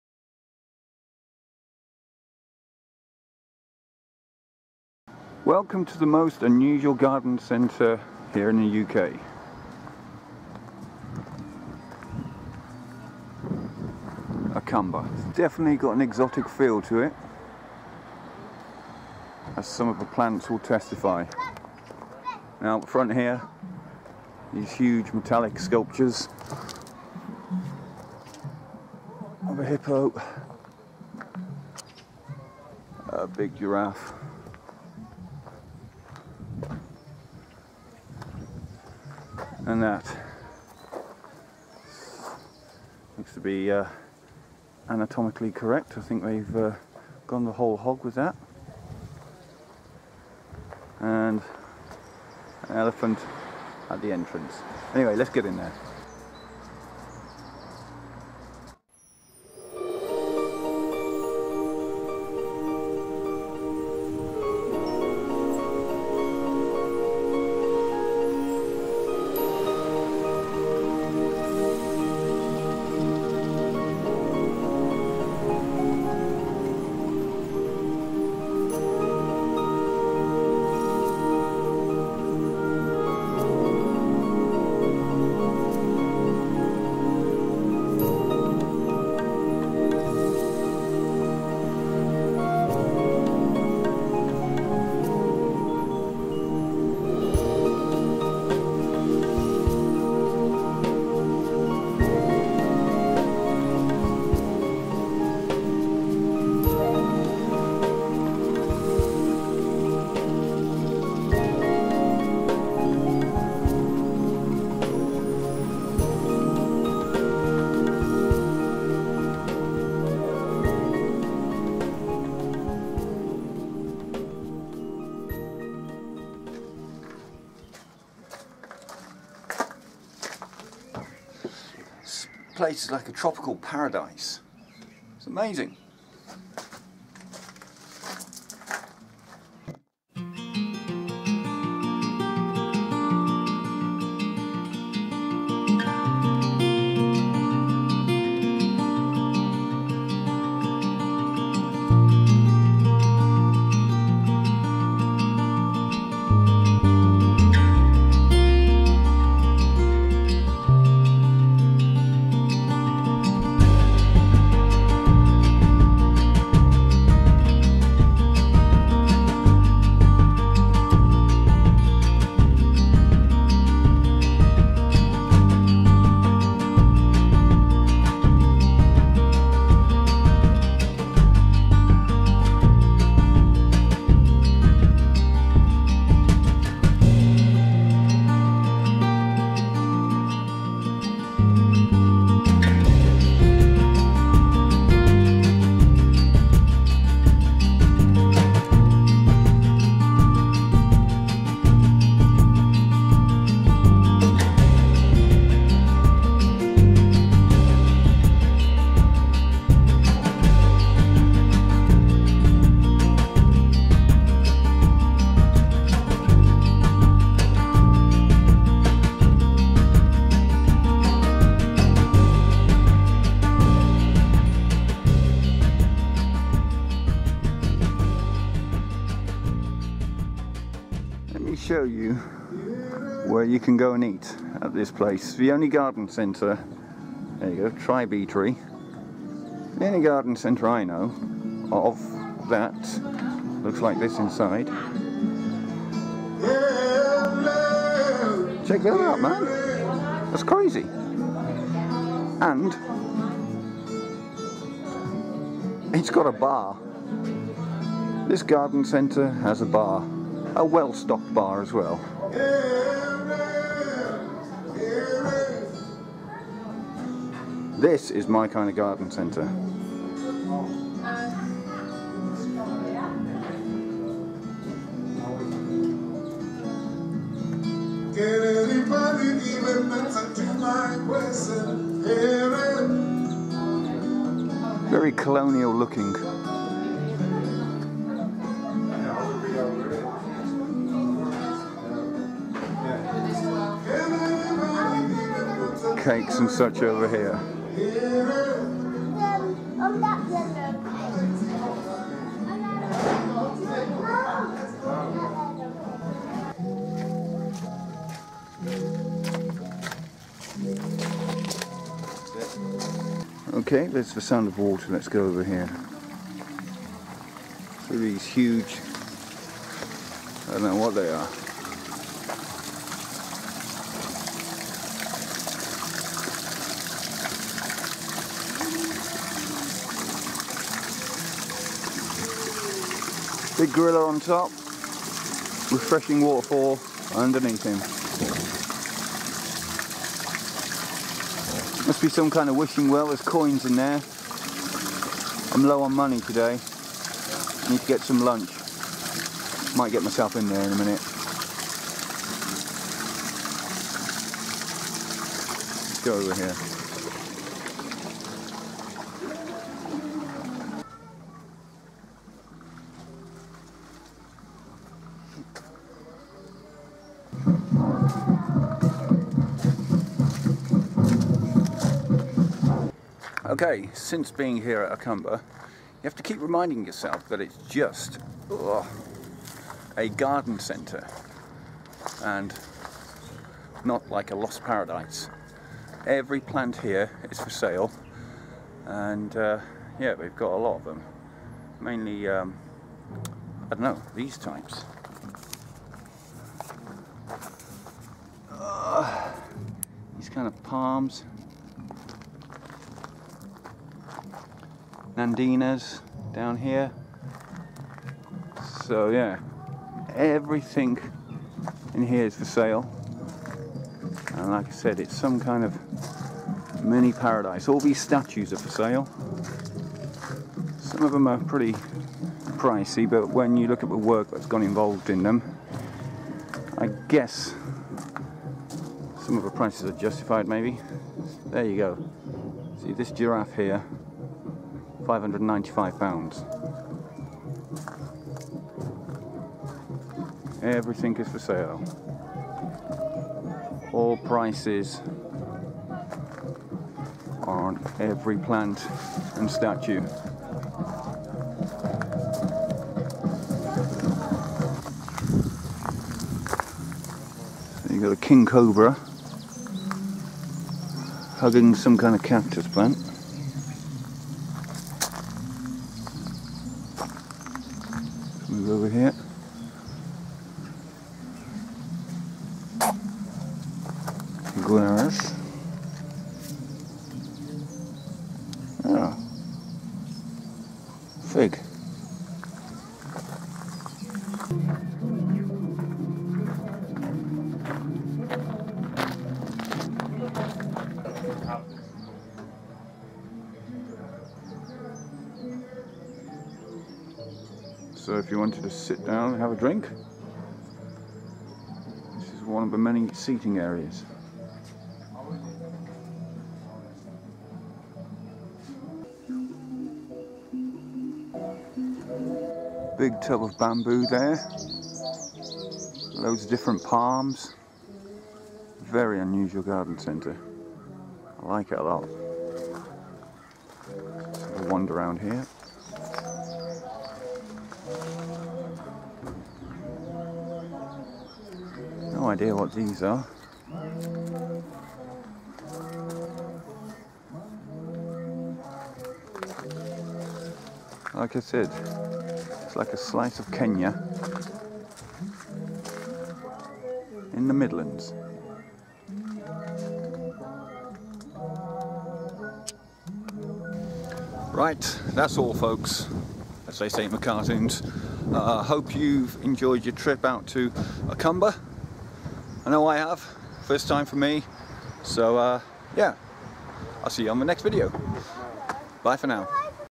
Akamba Garden Centre - Allotment Grow How
Something different for you today, I ventured out to Akamba Garden Centre for the first time. It was really interesting. I've tried to give you a brief taster of what the place looked like... but you really need to see it for yourself. Many Thanks